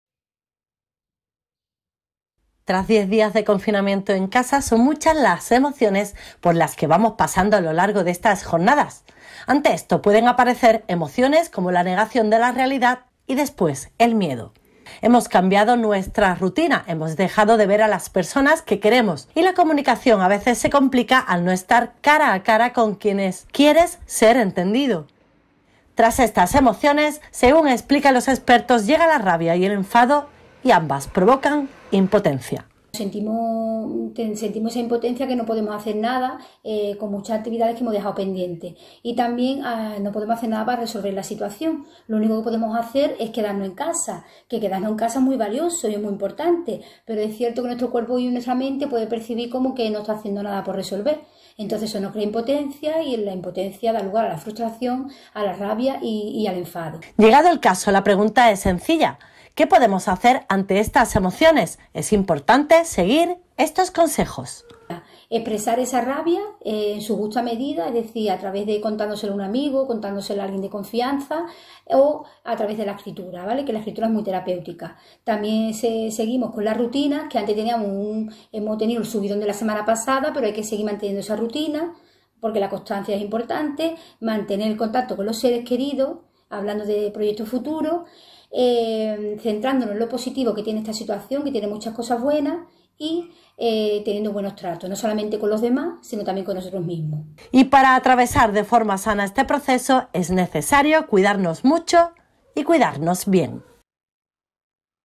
Tras dos semanas de encierro, son muchas las emociones por las que pasamos
Ante esto, pueden aparecer emociones como la negación de la realidad y después el miedo